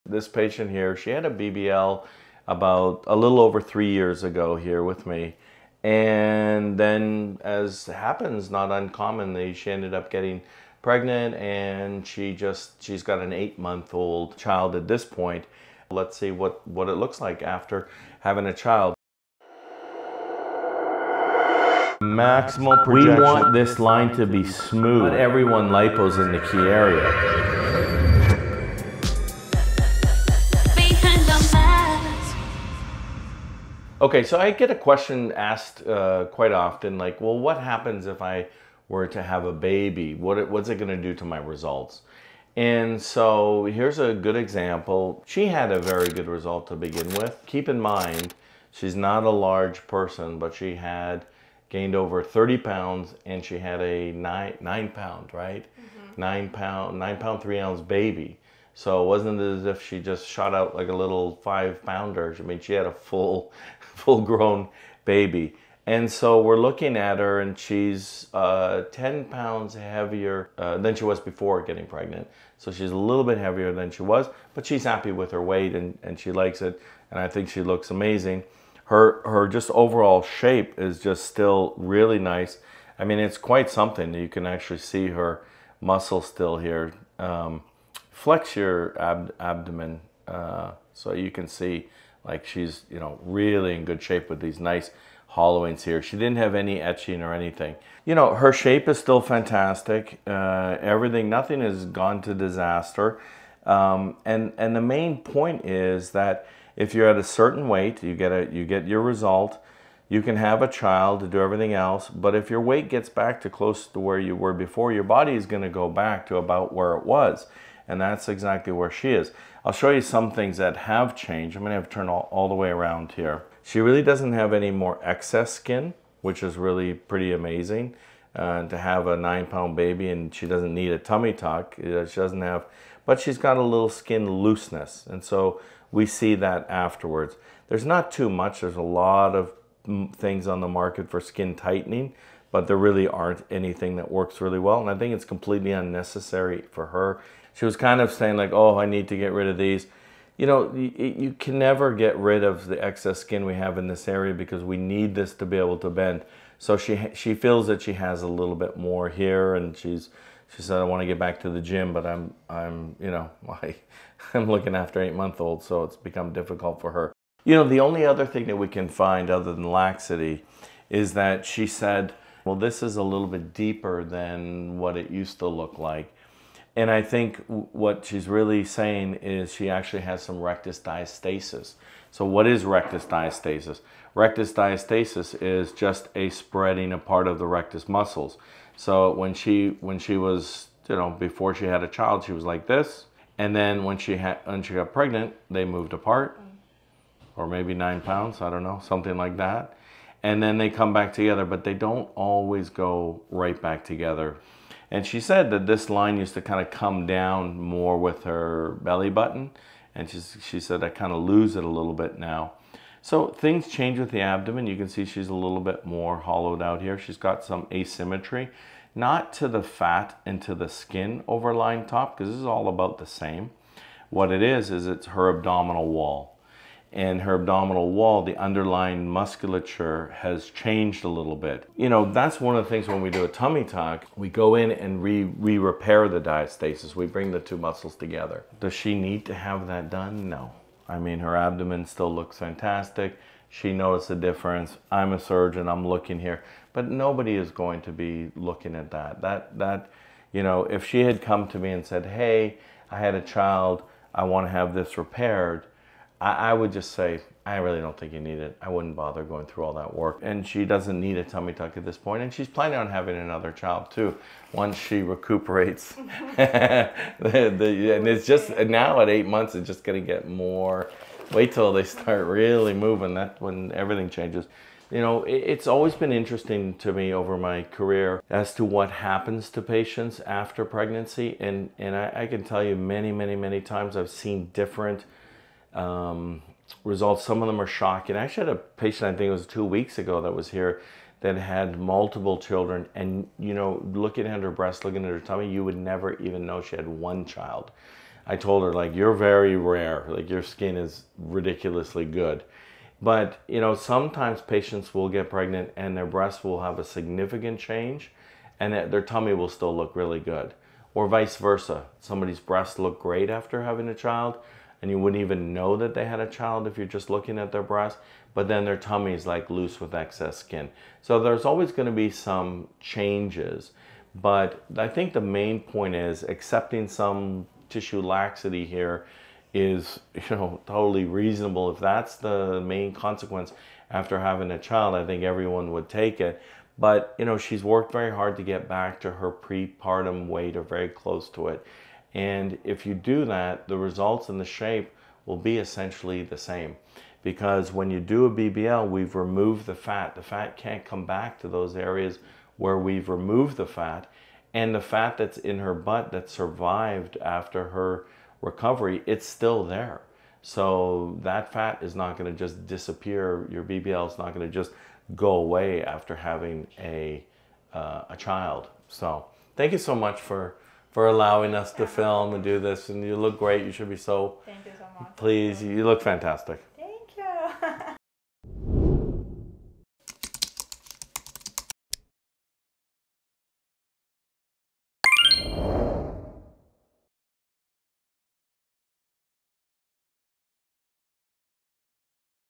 Pregnancy After a BBL: Am I Going To Loose My Results?
Sometimes you just don't want to wait until after having kids to have the body of your dreams, and many patients have concerns like whether they should wait until after pregnancy?  or will they lose their incredible BBL results? A Brazilian butt lift doesn't just give a new look— it may change the way you feel about yourself and boost your confidence. If you are dreaming of having curves like Beyonce, but also share concerns about when is the best time to do it, watch this new episode of Behind the Mask with Dr. William,  you will see the results of an actual patient who had a BBL 3 years ago and then decided to have a baby.

220 Alhambra Cir First Floor